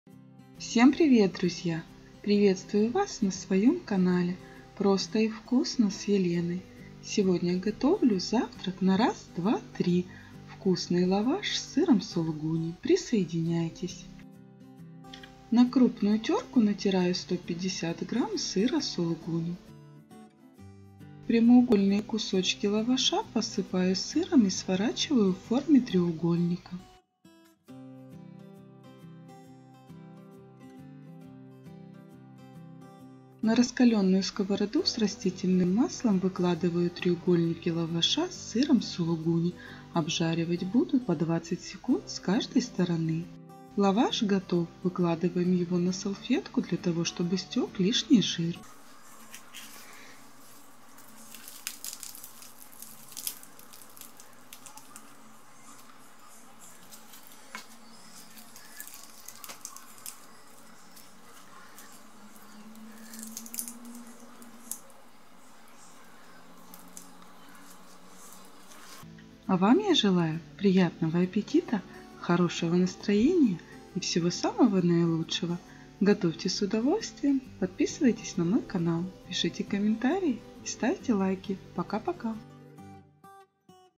Вот реально-проще не бывает! Конвертики из лаваша с сыром сулугуни
Предлагаю Вашему вниманию рецепт приготовления вкусного завтрака либо перекуса-Конвертиков из лаваша с сыром сулугуни. Очень быстрый и легкий в приготовлении рецепт из лаваша с сыром!

Ингредиенты
2 лаваша
150 гр сыра сулугуни
Растительное масло для обжарки

Желаю Вам приятного аппетита, хорошего настроения и всего самого наилучшего!